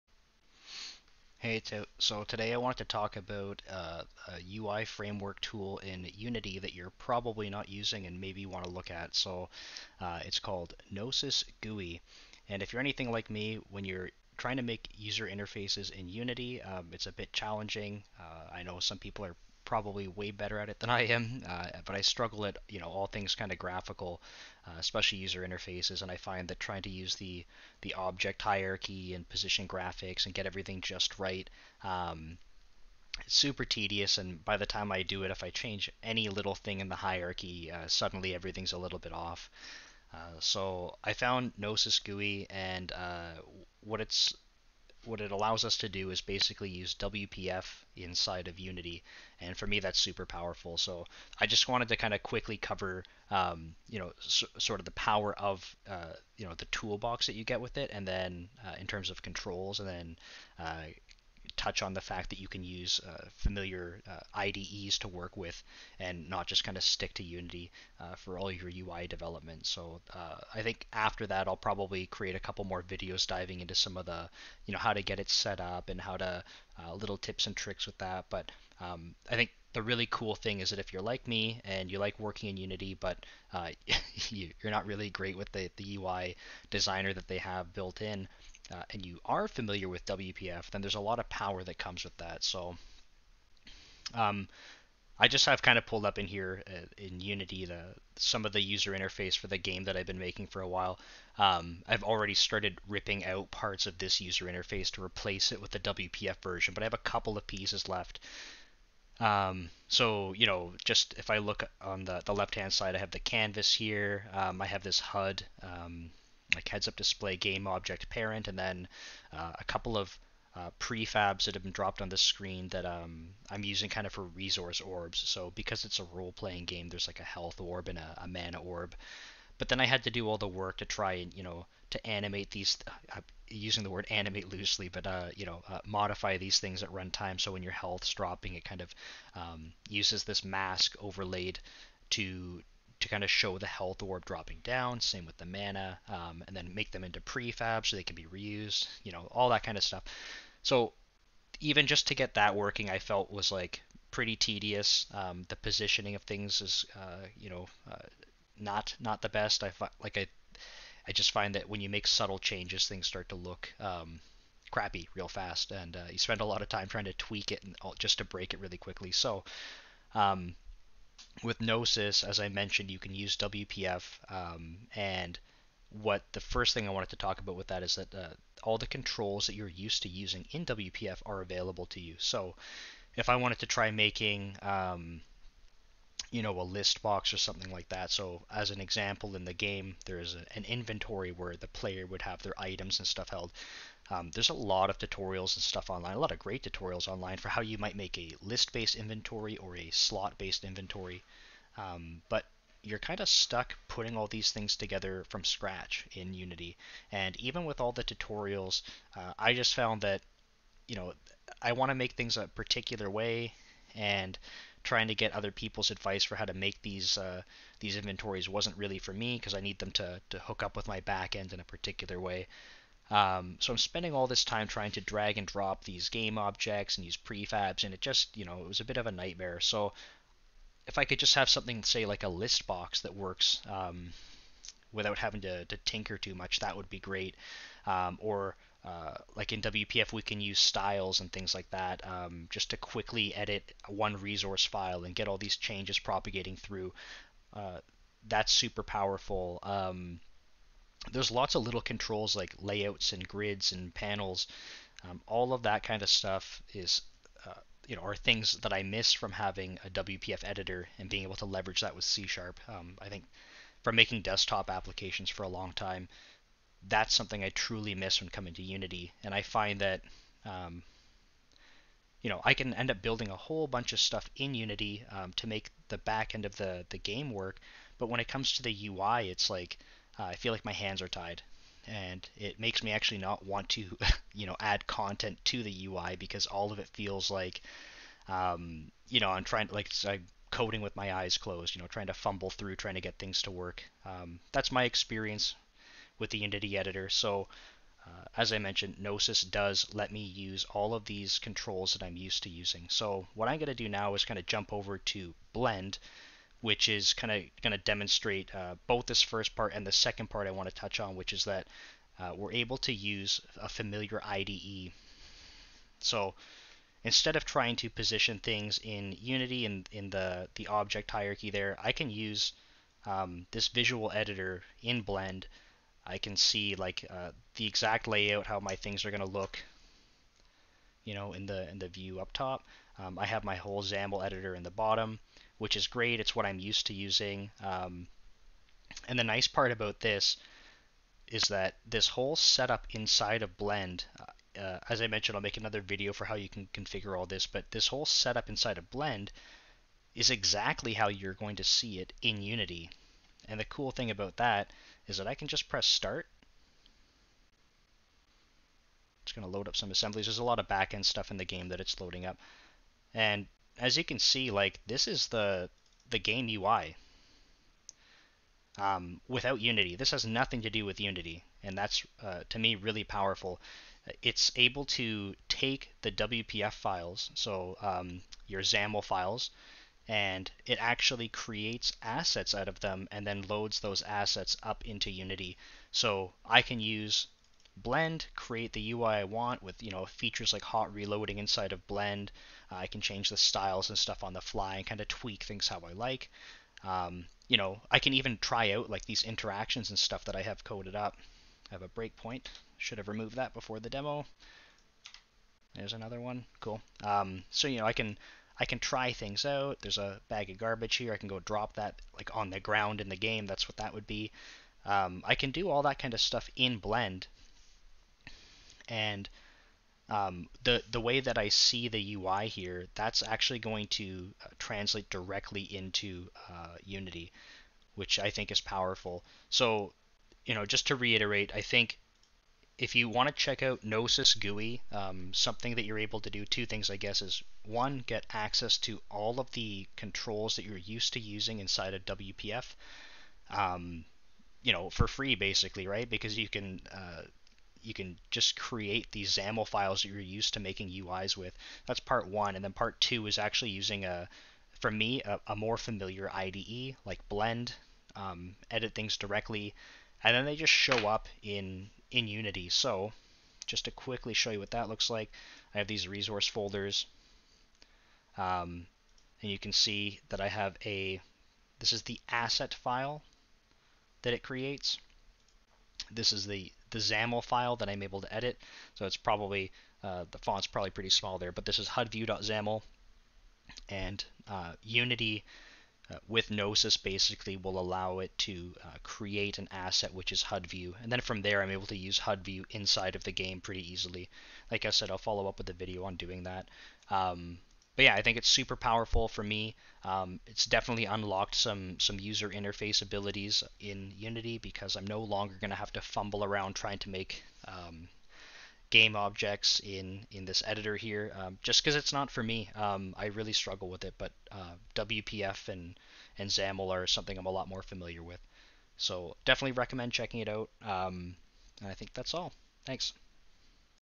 The Unity UI Framework That You Probably Aren't Using!
In this video I introduce a lifesaver for me in terms of Unity3D GUI development... NoesisGUI! More videos to follow on how you can get up and running using NoesisGUI but in the meantime, check out: